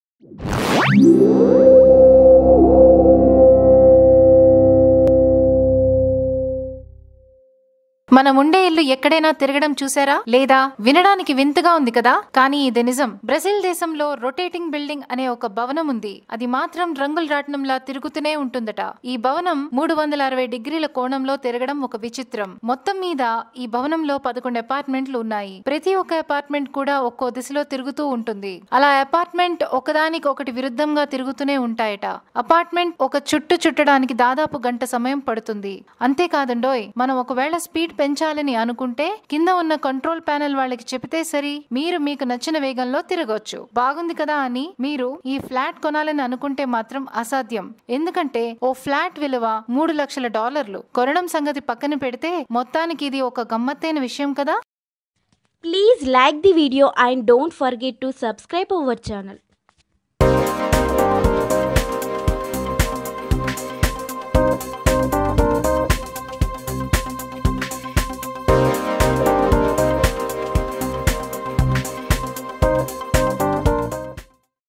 రొటేటింగ్ బిల్డింగ్...! ఈ బిల్డింగ్ గురించి వింటే ఆశ్చర్యపోతారు.. | Ninth Secret
Not only earth, even your BUILDING is rotating!